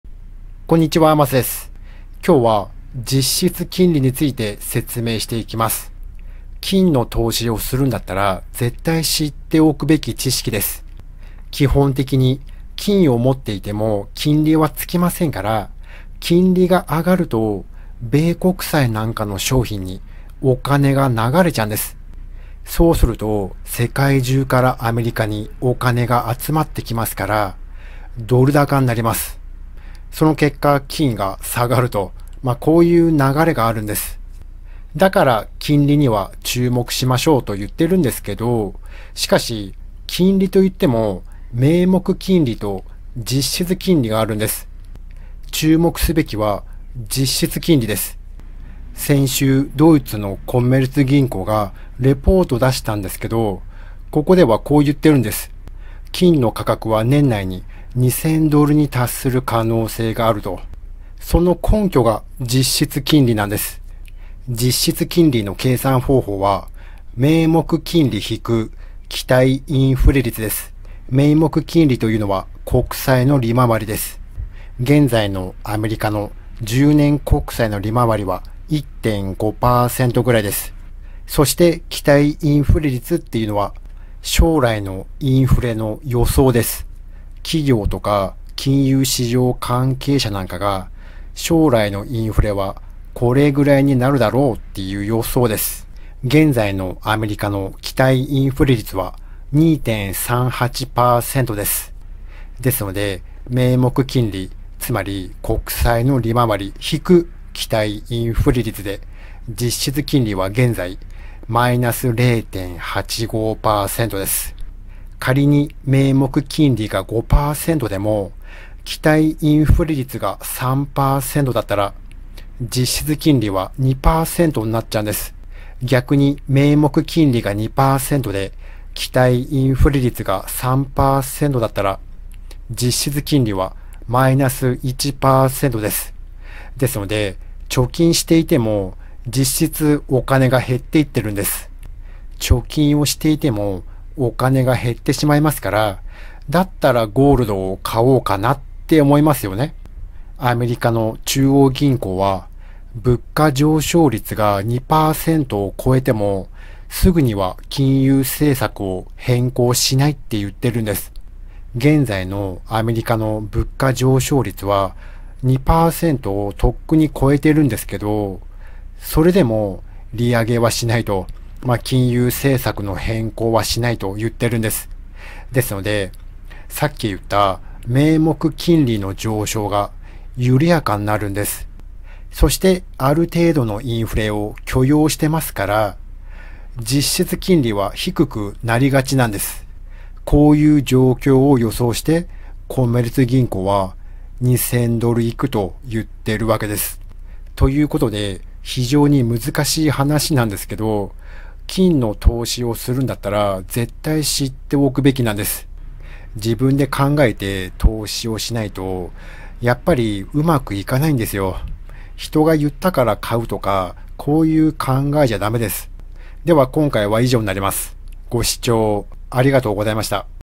【実質金利マイナス】コメルツ銀行の金価格予測=年内2000ドル
コメルツ銀行がゴールド価格の予測を発表しました。その根拠は実質金利のマイナスが長い間続くと考えているからです。